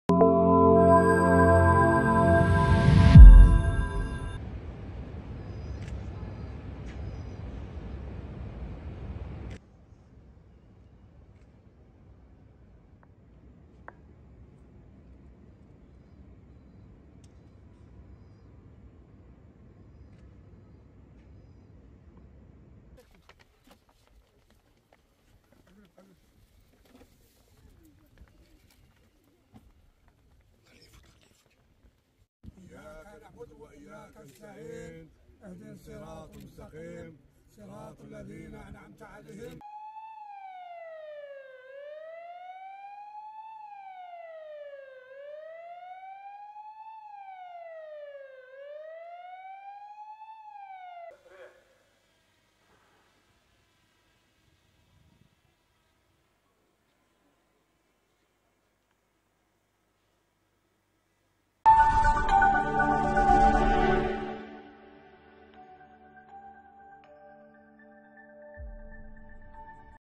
وقفة ترحم عبر الوطن على أرواح شهداء مجازر 17 أكتوبر 1961
الجزائر (وأج) - تم اليوم الثلاثاء على الساعة ال11 صباحا  عبر مختلف ربوع الوطن  تنظيم وقفة ترحم على أرواح شهداء مجازر 17 أكتوبر 1961.وتأتي هذه الوقفة  تنفيذا لقرار رئيس الجمهورية, السيد عبد المجيد تبون, سنة 2021, بالوقوف دقيقة صمت عبر كامل ربوع الوطن لإحياء هذه المناسبة التي تتزامن مع اليوم الوطني للهجرة.